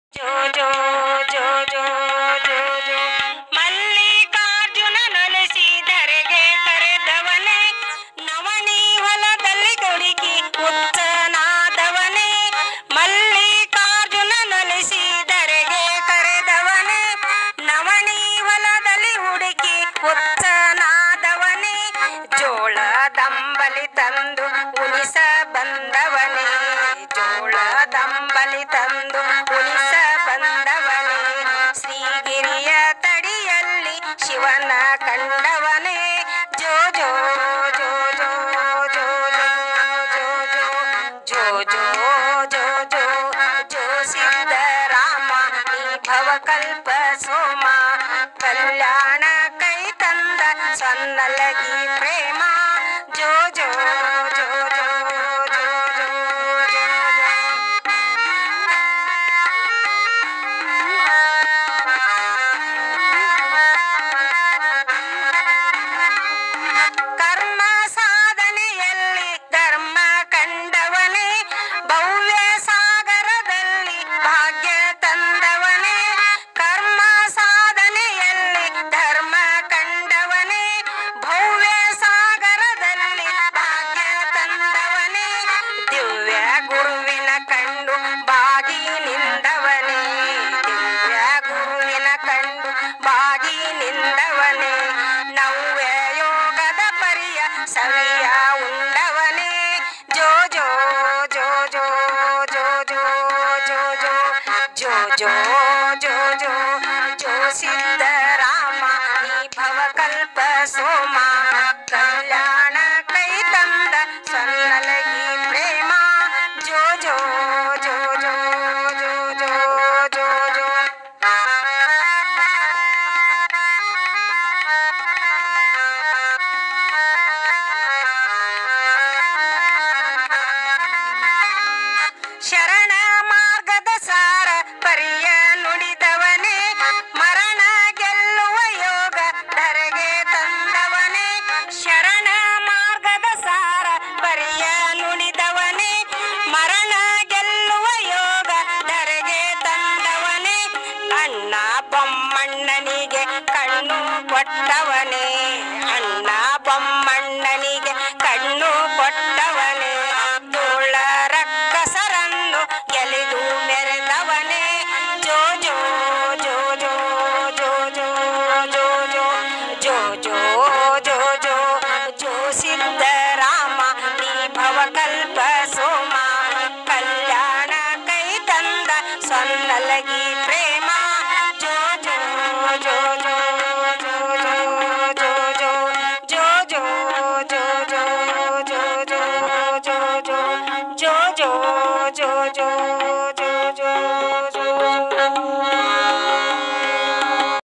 My mother is singing cradle song and I'm playing key board
On Shri Basava Tv